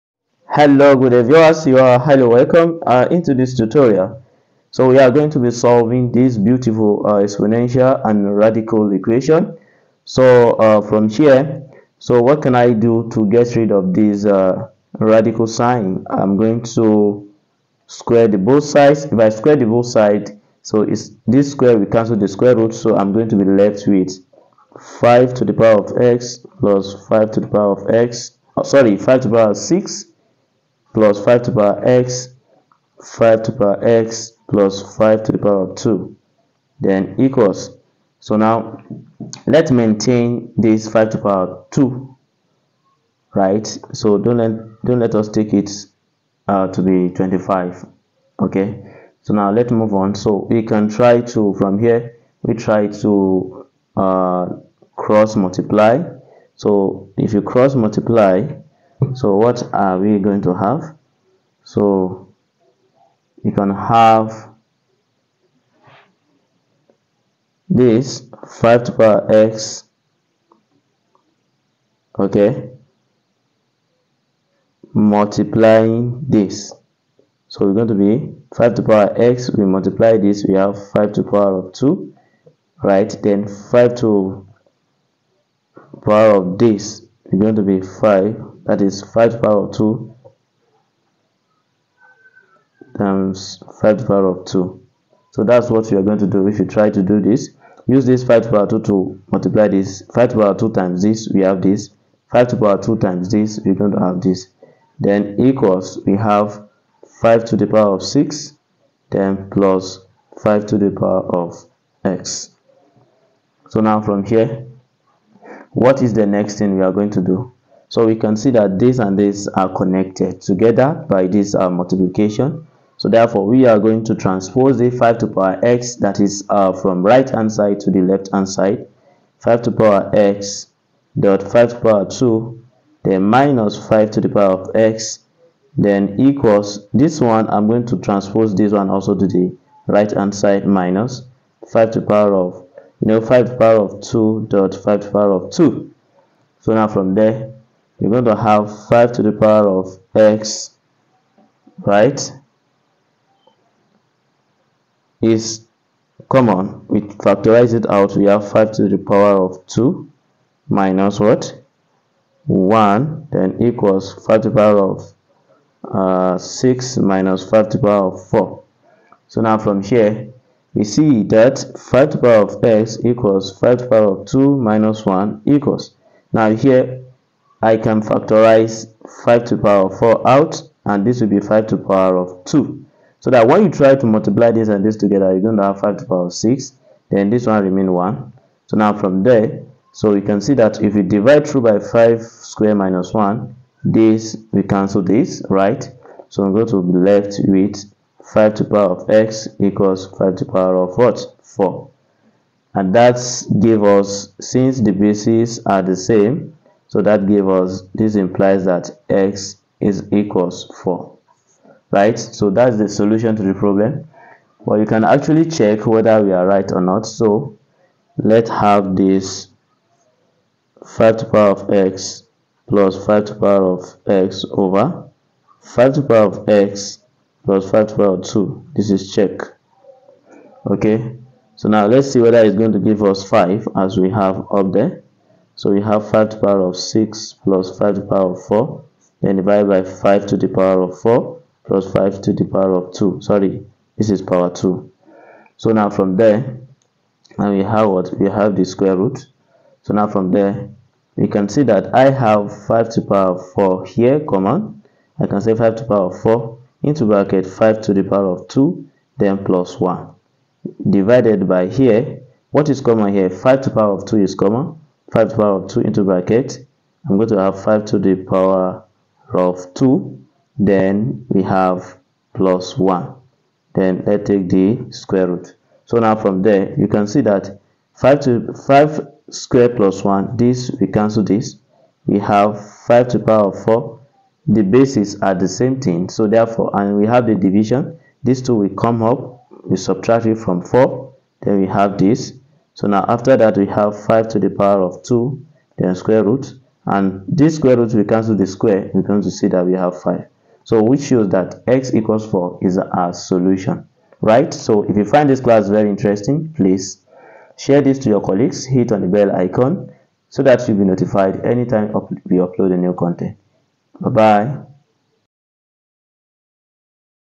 INDIA | Mastering Radical and Exponential Equations: Step-by-Step Tutorial
In this video, we dive deep into solving radical and exponential equations, providing a clear and comprehensive tutorial for students and math enthusiasts in India.

We'll cover essential techniques, step-by-step solutions, and tips to tackle even the trickiest problems. Whether you're prepping for exams or enhancing your math skills, this video is your go-to resource.